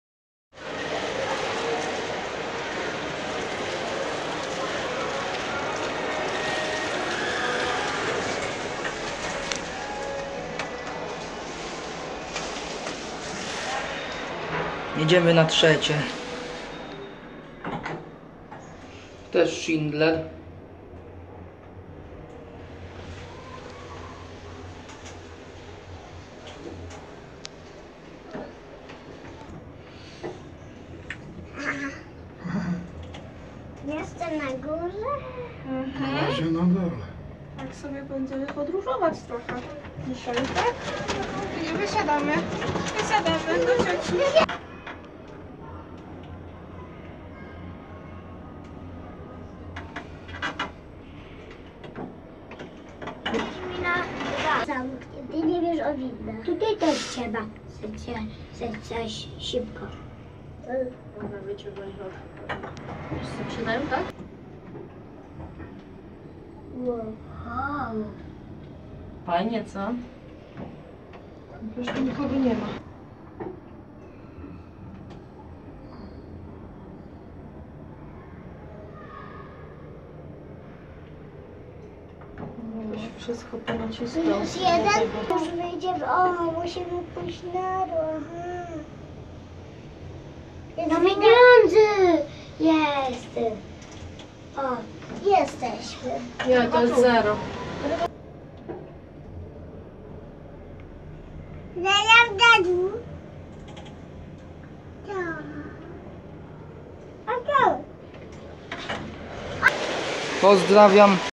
Główny Schindler EuroLift, Galeria Rzeszów
Piętra: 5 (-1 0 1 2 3)
Modernizacja: Nie
Prędkość: 1m/s
Rok budowy: 2012
Typ windy: Osobowy
Firma montująca windę: Schindler